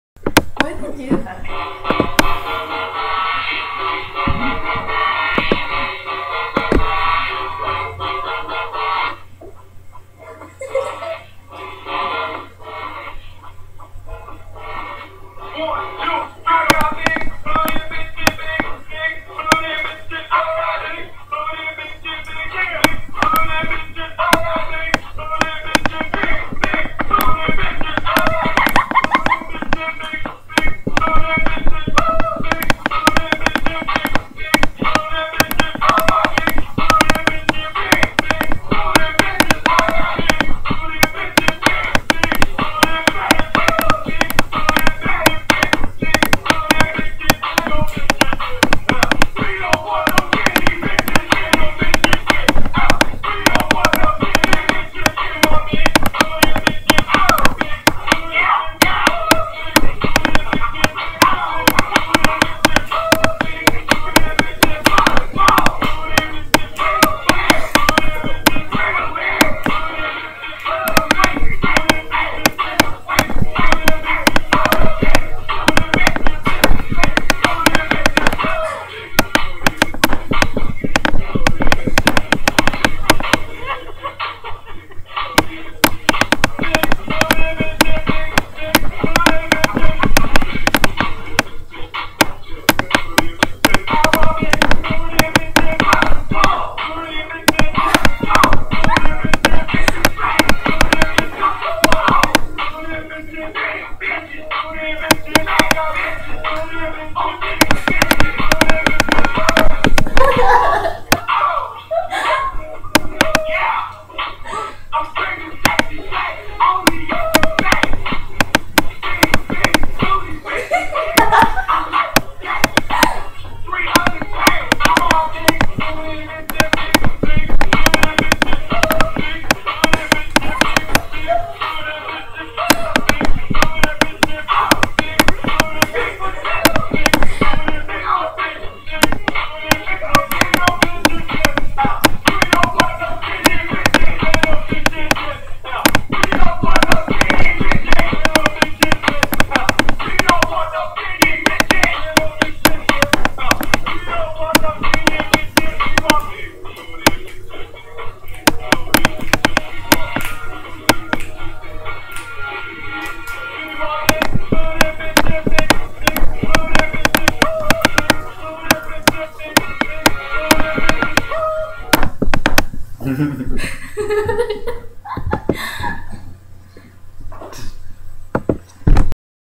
big bOOty silly dance
hehe that first laugh you hear is not me! its from the big booty orignal videooo (;  sorry for thhe popping noise should have put the laptop on a table.